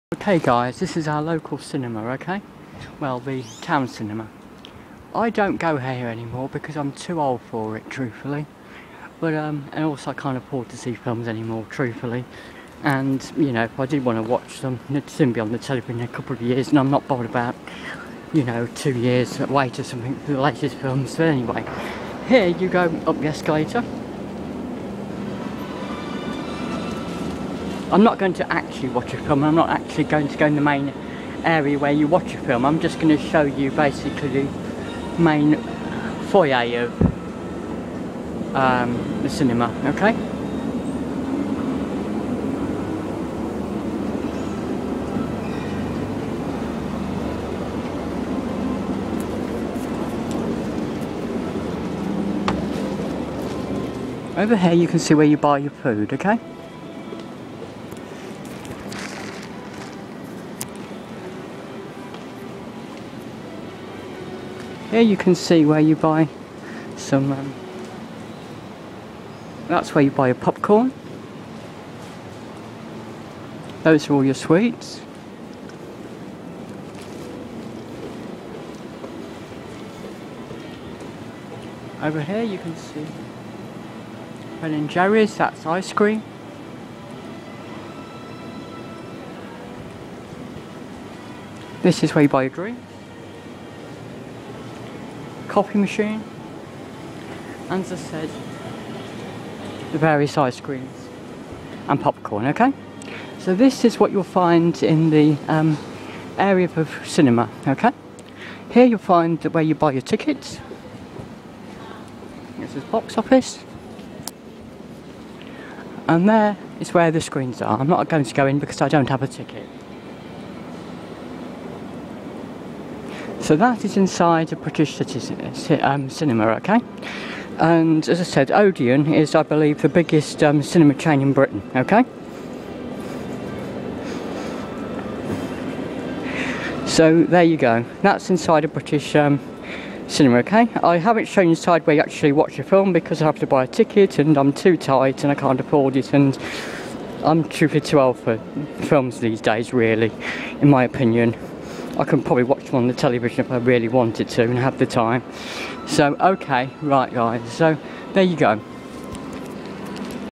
Cliff Shows You around - Coventry Skydome Odeon Cinema Foyer
This was done quickly as the battery was running out. I am aware it does not seem very professional. It was done for foreigners to see inside a British cinema.
I only show the food area and where they tickets are bought, not the seating area where you watch films etc.